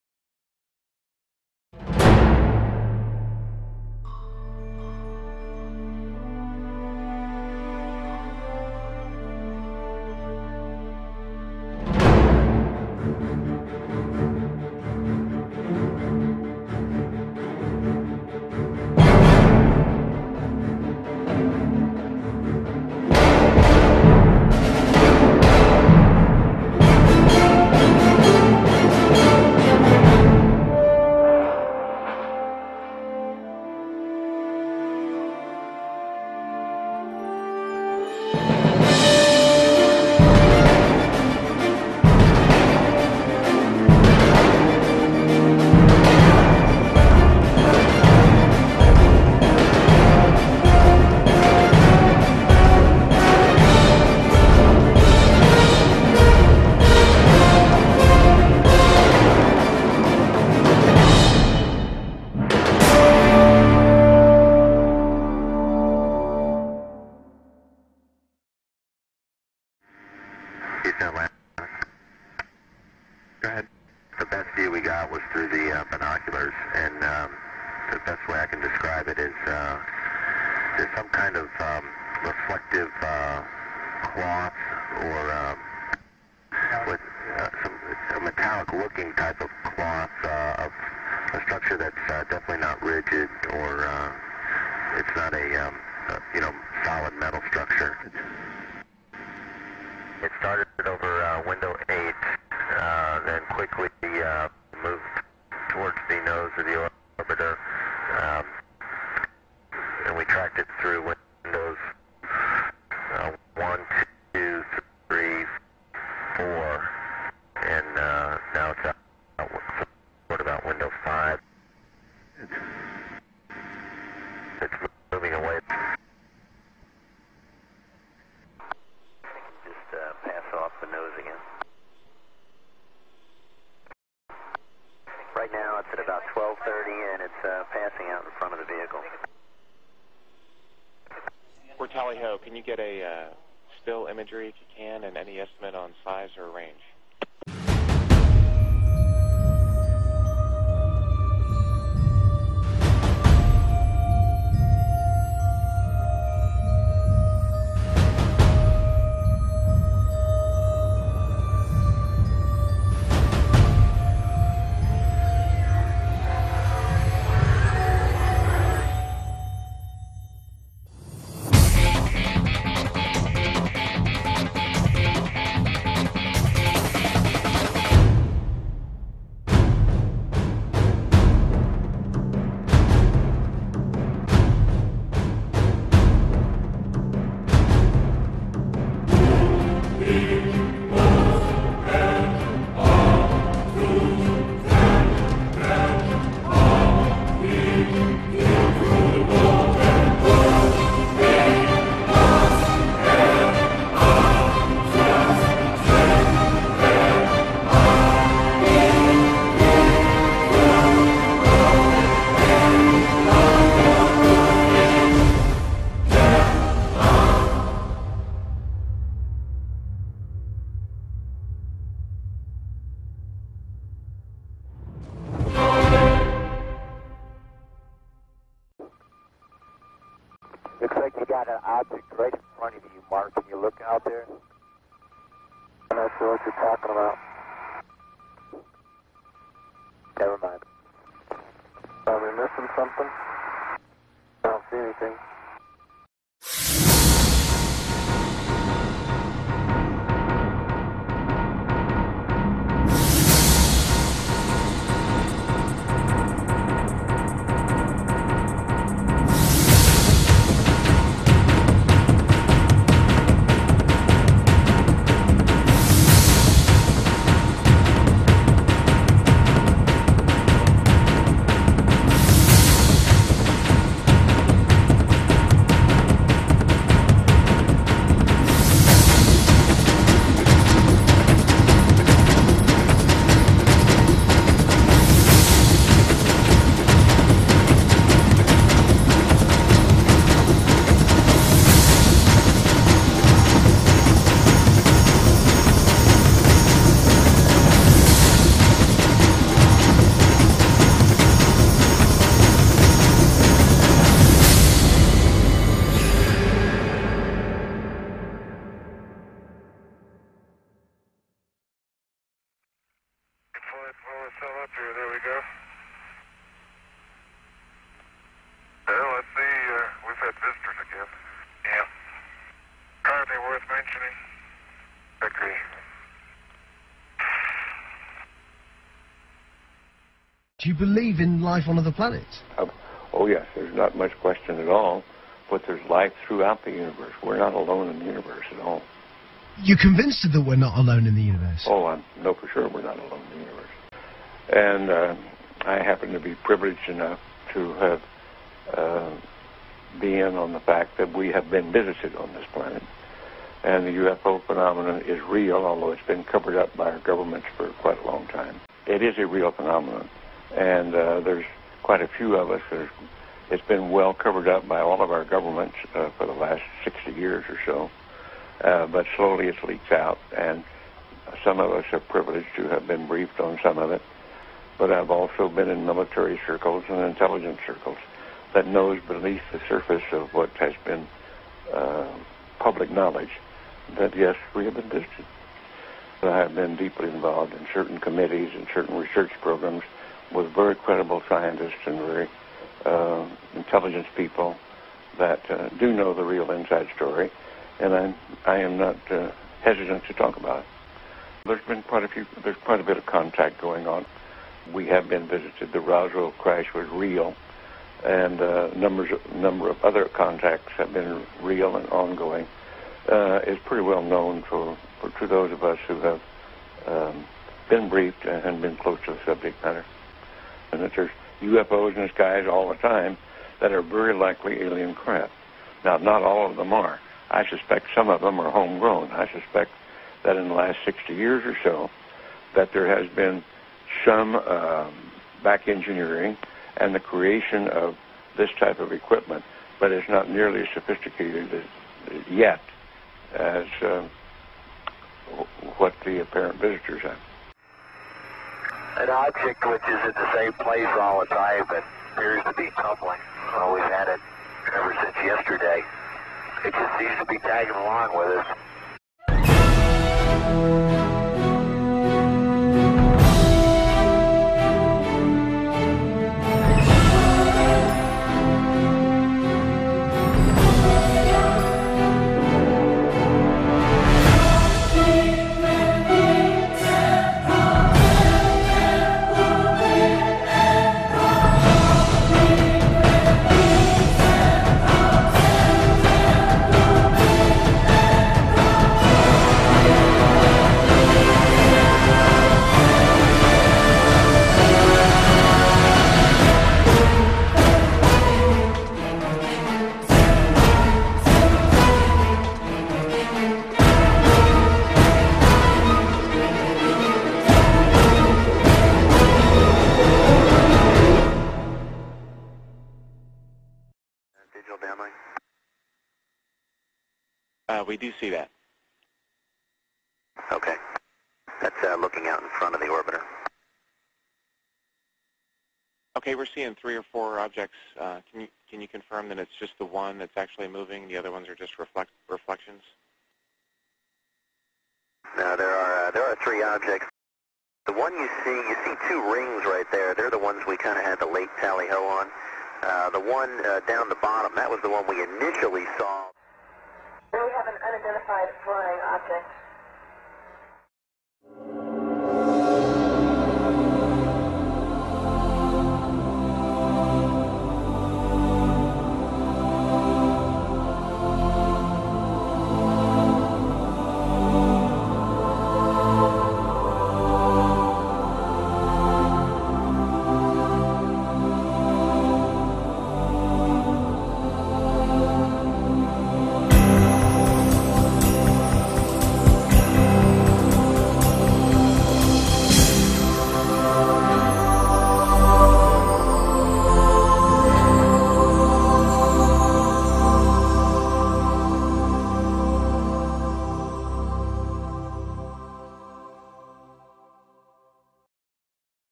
UFO in the NASA Archives
NASA footage of ufo and astronauts interviews confirming sightings in space.